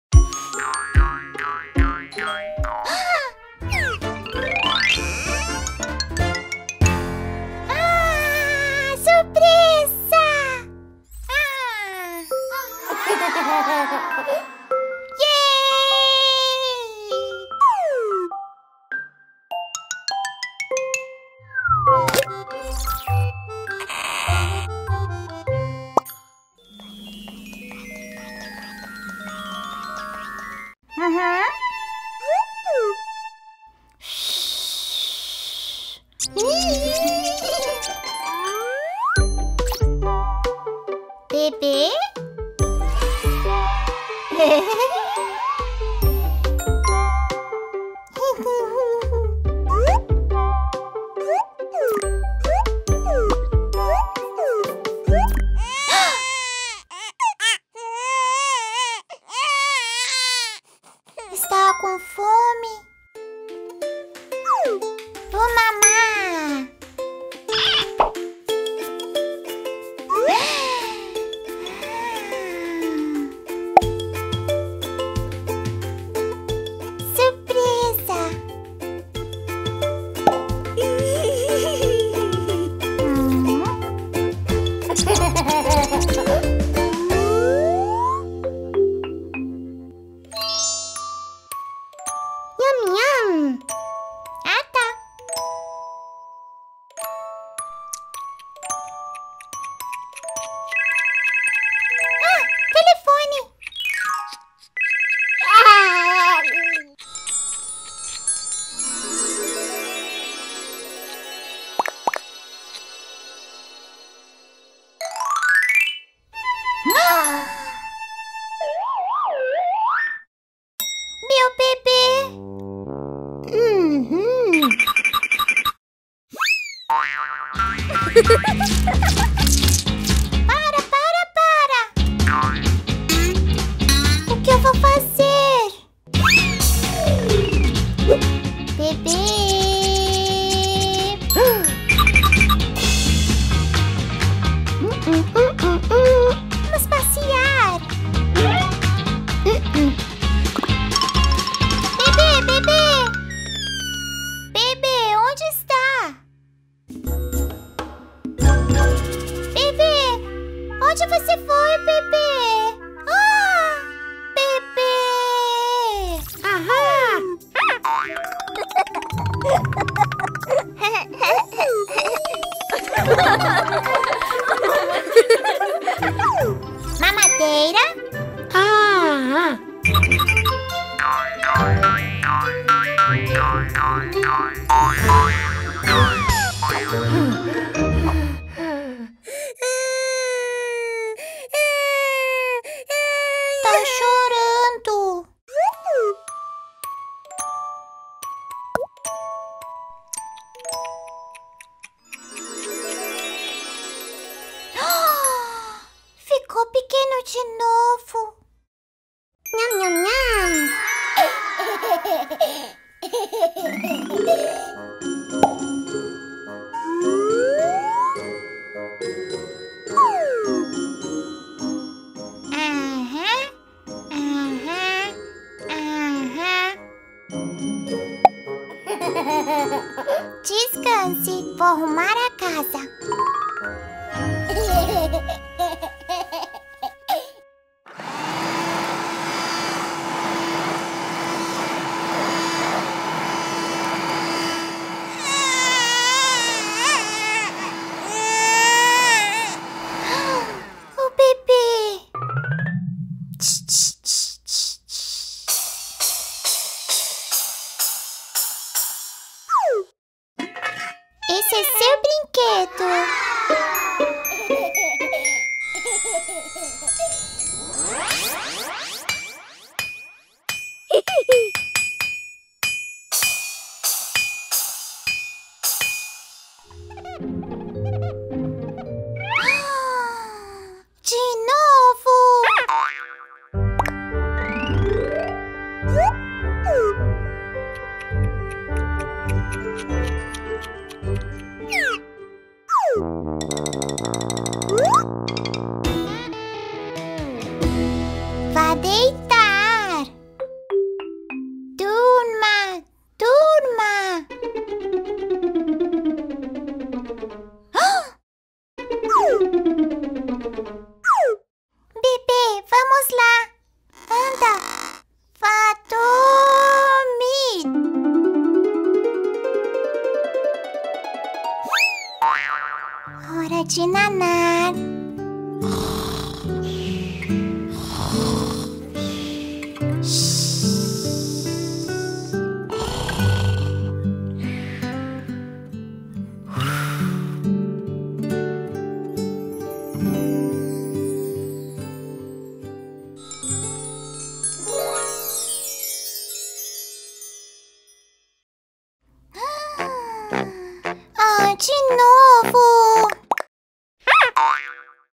Diana e o boneco travesso
Diana ganha um boneco novo de presente. Entretanto, quando Diana deixa o boneco sozinho, ele transforma-se no Roma e começa a fazer travessuras!
Diana brinca com um boneco bebê e com brinquedos de criança